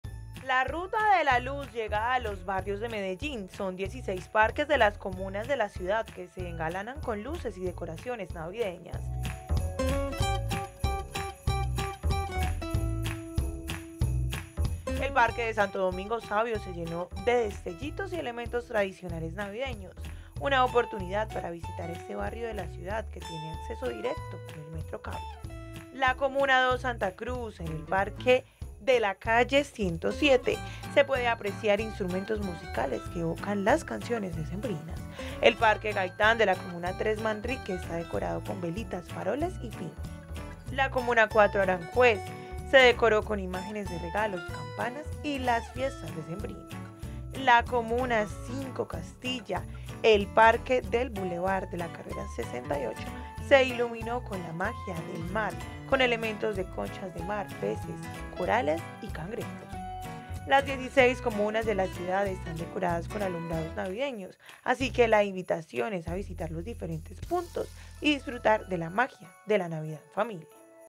El plan para ver los alumbrados en Medellín - Telemedellín
La ruta de la luz llega a los barrios de Medellín, son  16 parques de  las comunas de la ciudad  que se engalanan con luces y decoraciones navideñas.

El Parque de Santo Domingo Sabio se llenó de destellitos y elementos tradicionales navideños, una oportunidad para visitar este barrio de la ciudad que tiene acceso directo con el Metrocable.